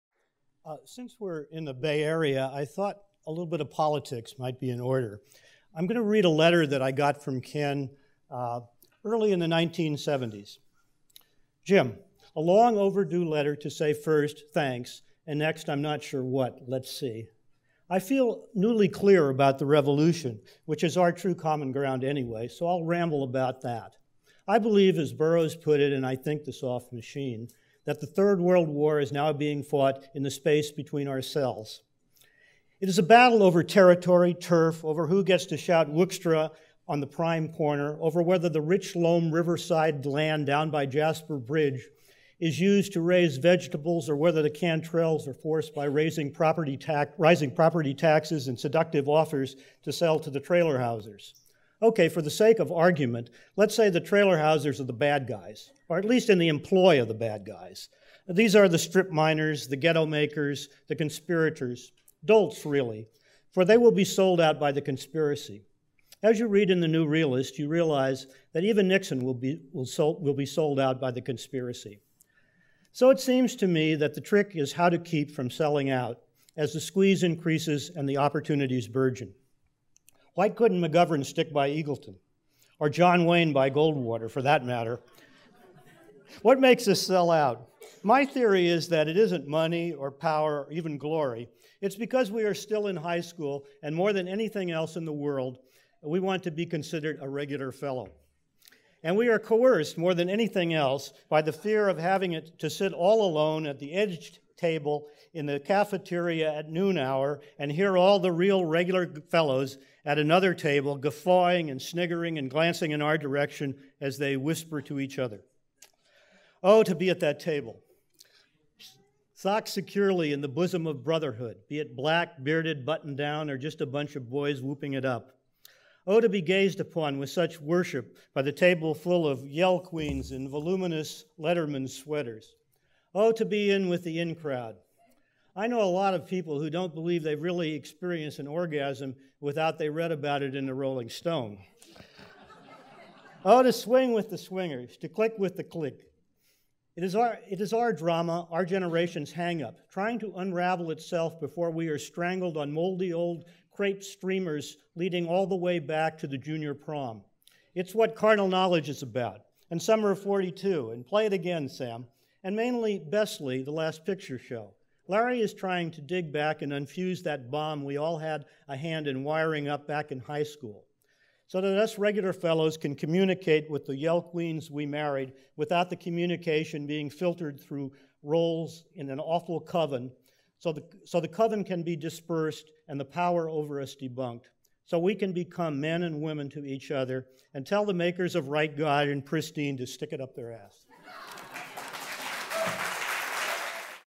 2001 Ken Kesey Tribute by Jim Wolpman at the San Francisco Public Library
Labor attorney Jim Wolpman was one of the Perry Lane neighbors of Ken Kesey in Palo Alto, California. On December 5, 2001, he read an imaginative 1970's letter from Kesey at his memorial. Check out Ken Kesey's books in our online catalog